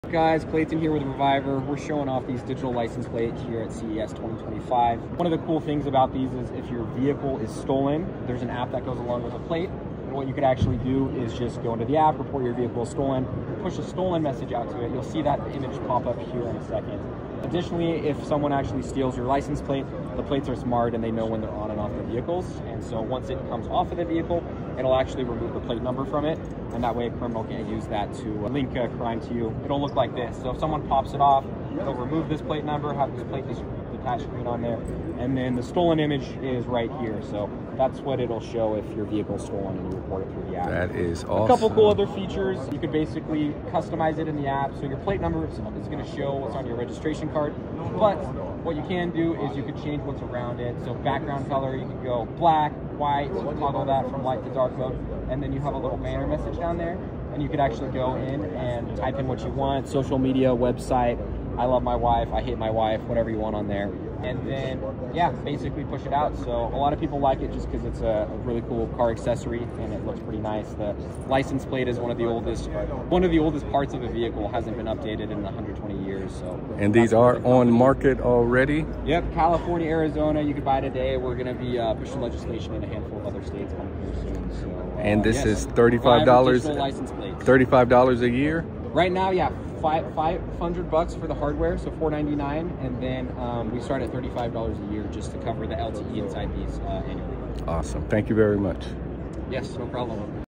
Digital License Plates: The Car Upgrade You Need
Digital license plates are getting an upgrade that's revolutionizing how we think about vehicle identification! Straight from CES 2025, @ReviverRPlate smart plates are changing the game with incredible security features and customization options.

Want to protect your car from theft? These plates instantly display a "STOLEN" message through the app if your vehicle gets taken. Plus, they're tamper-proof - if someone tries to remove the plate, it automatically erases the number to prevent misuse in crimes.

Customize your ride with sleek black or white backgrounds and add personal messages below your plate number. Currently available in California and Arizona, these plates come with built-in LTE connectivity for just $35/year after the initial hardware cost or only $499

Ready to upgrade your car with the first major license plate innovation in 120 years? This smart plate does more than just look cool - it's bringing vehicle identification into the future with style and security.